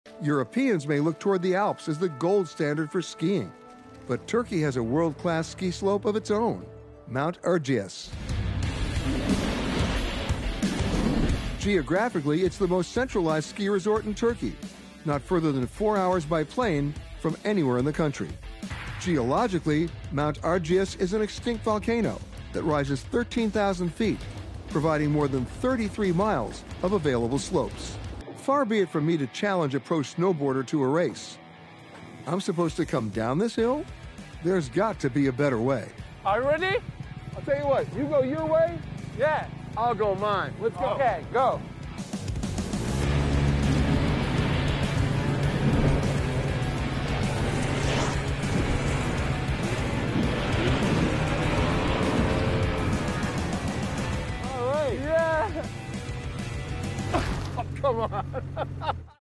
Erciyes Dağı
Let’s explore the stunning snowflakes of ErciyesMountain in Kayseri.
HiddenTurkey will lead you to witness the worldwide gold standard for skiing in Kayseri as @petergreenberg 🎥 experiences this amazing ski resort.